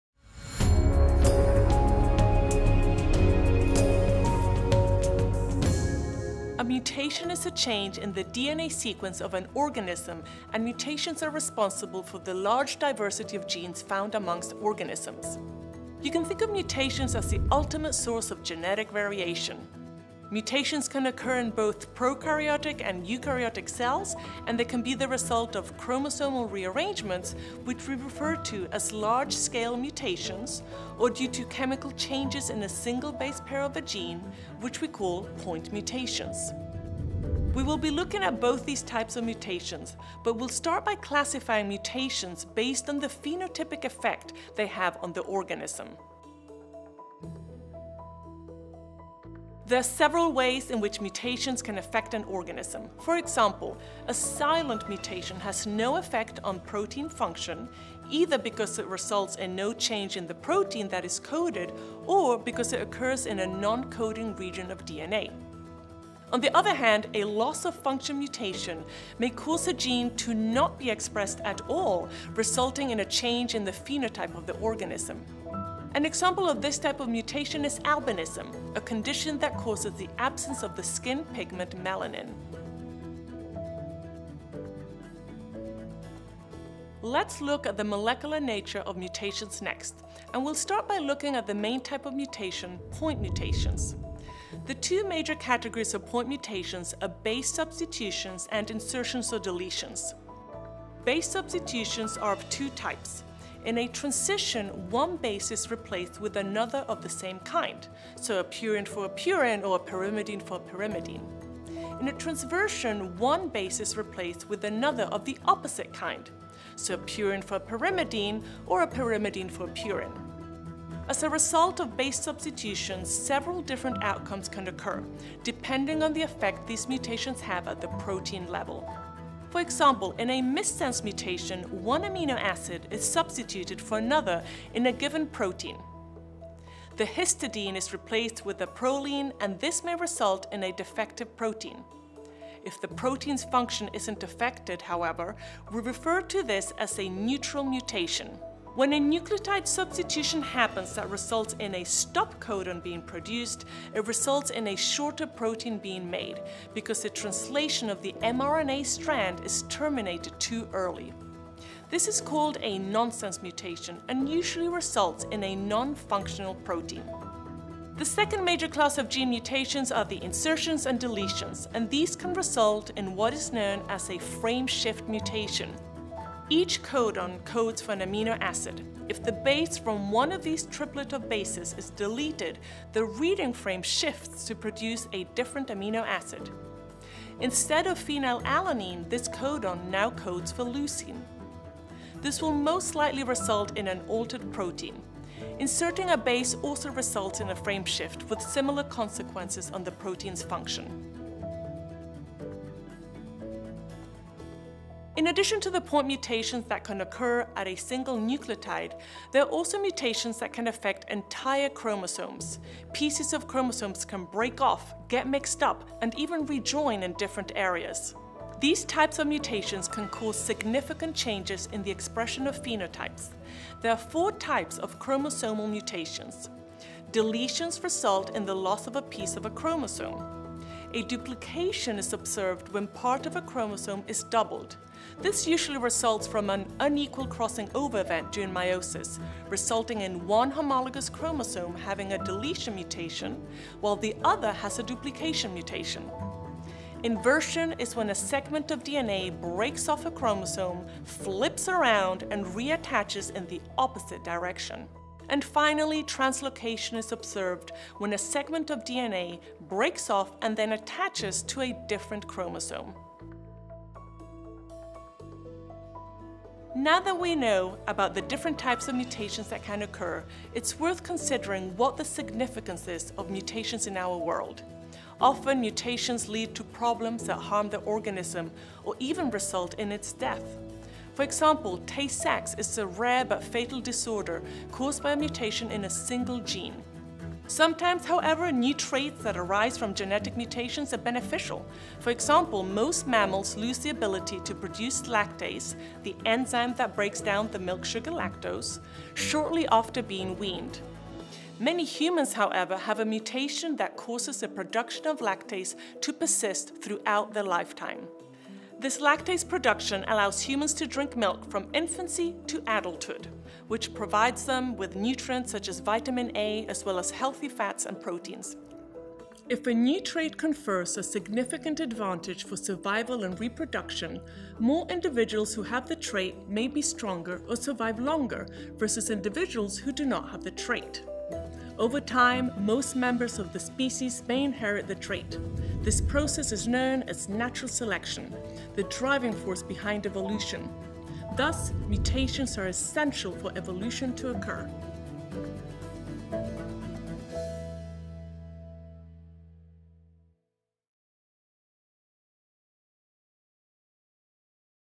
Mutations | Biology
Summarize videos instantly with our Course Assistant plugin, and enjoy AI-generated quizzes Learn all about biological mutations in just a few minutes! Jessica Pamment, professional lecturer at DePaul University, explains the process by which DNA is used in conjunction with different types of RNA to build proteins.